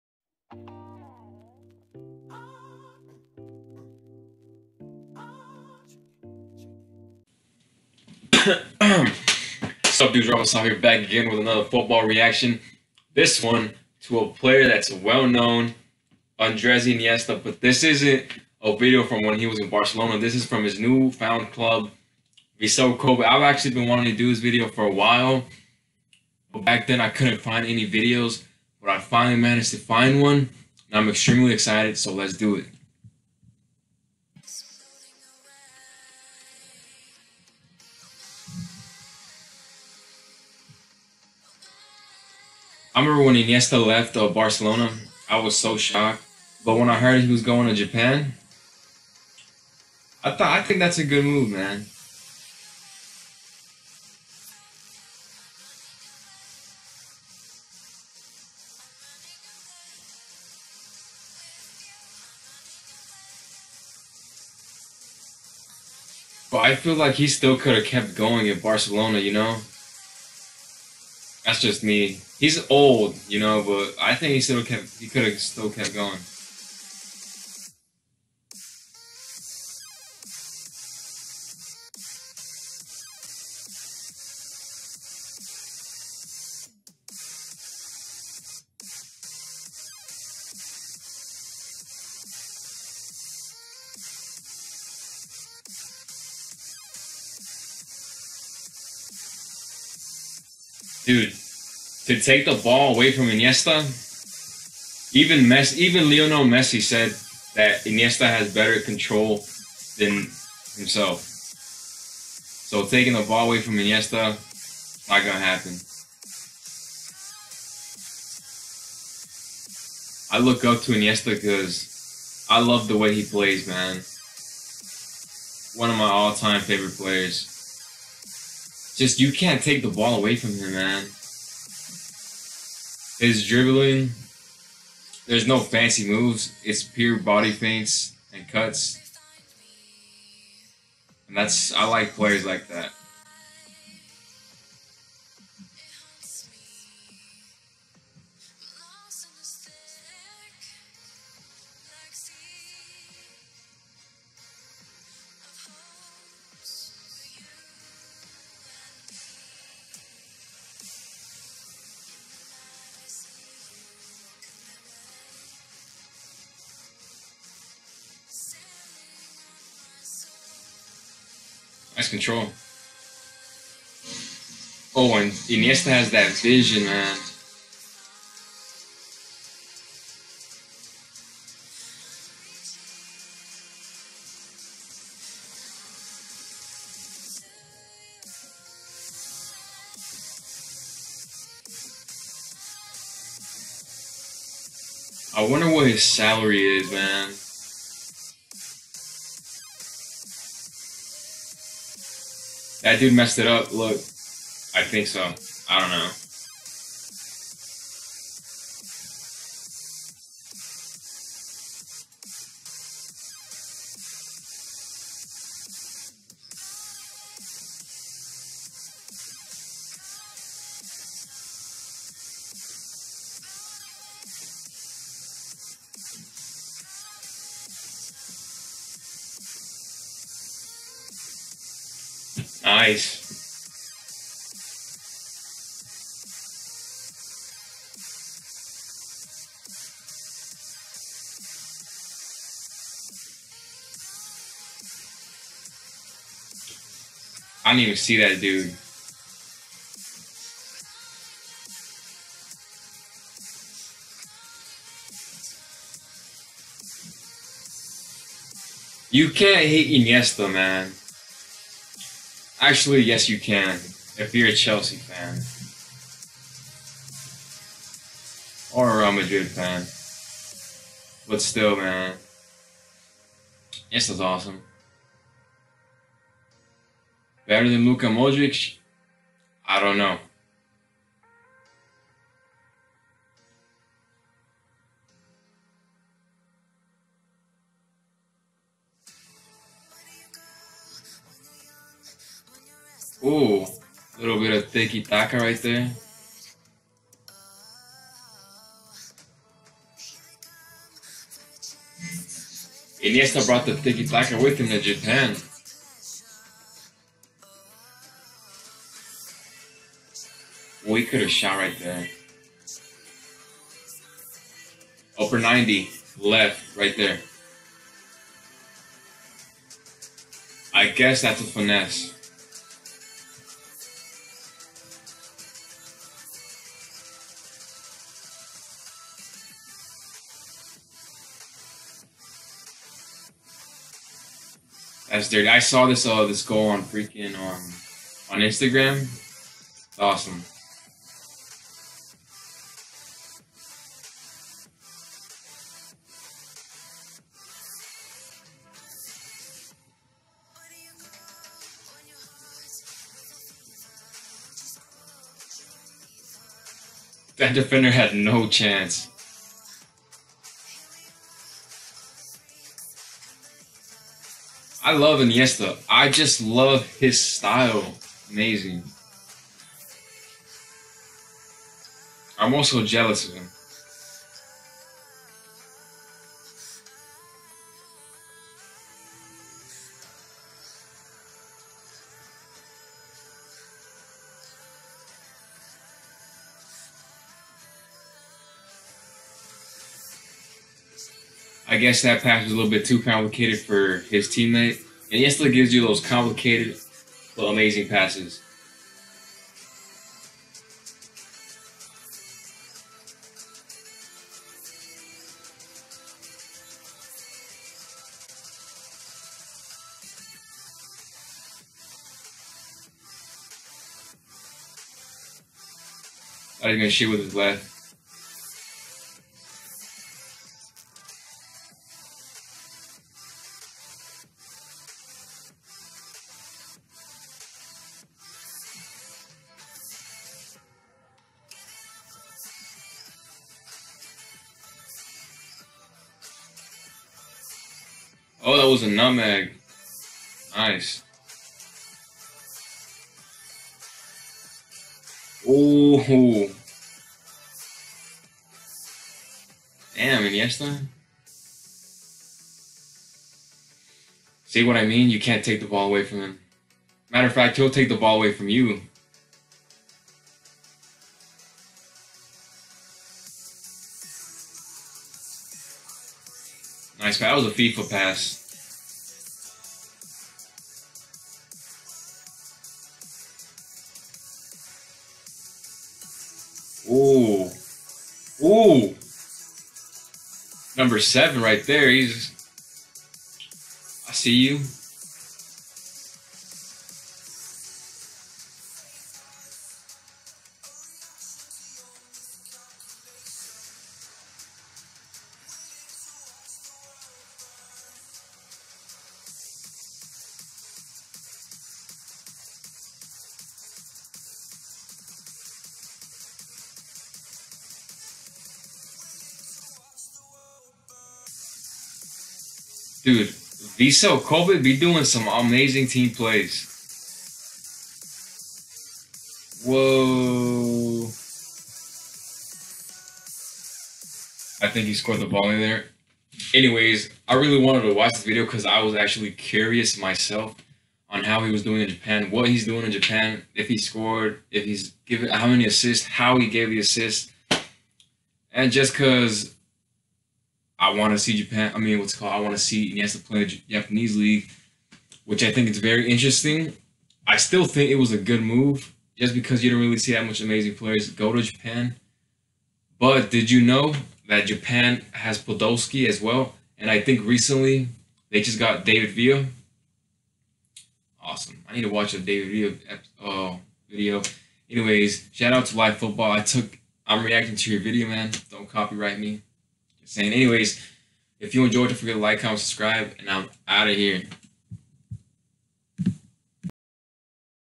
INIESTA in JAPAN 2018 Amazing Skills - Assist & Goals Vissel Kobe REACTION
INIESTA in JAPAN 2018 Amazing Skills - Assist & Goals Vissel Kobe
Finally a video of Iniesta in Japan!!